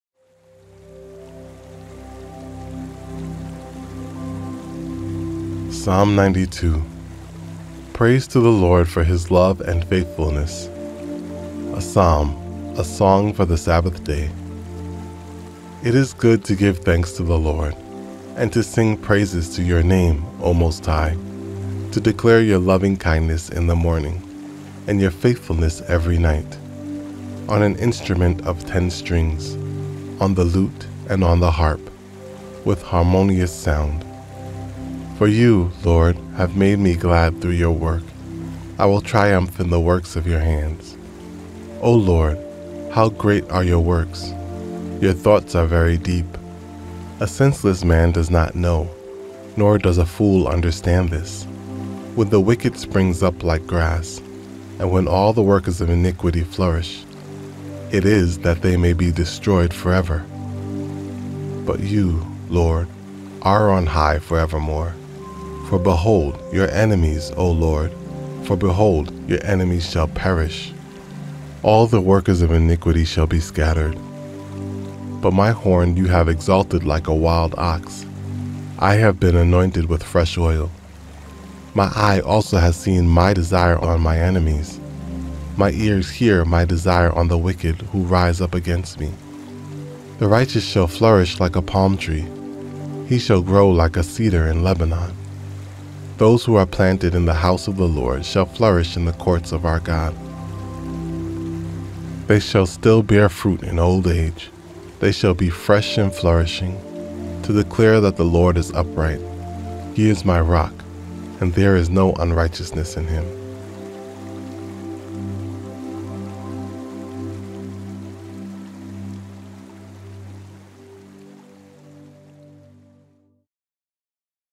Psalm 92 (NKJV) - Praise to the Lord for His Love and Faithfulness
Praise to the Lord for His Love and Faithfulness

A Psalm. A Song for the Sabbath day.